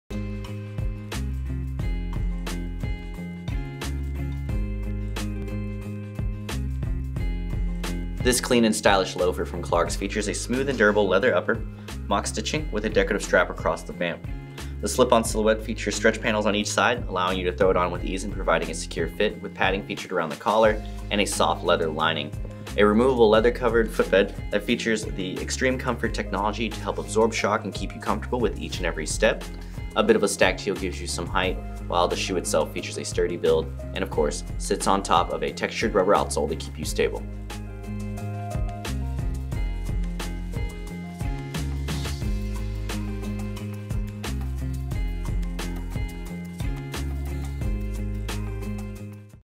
Clarks Whiddon Loafer SKU: 9476384
Clarks&reg; Whiddon Loafer.
Slip-on design.
Leather upper.
Leather and textile lining and insole.
Synthetic outsole.
Imported.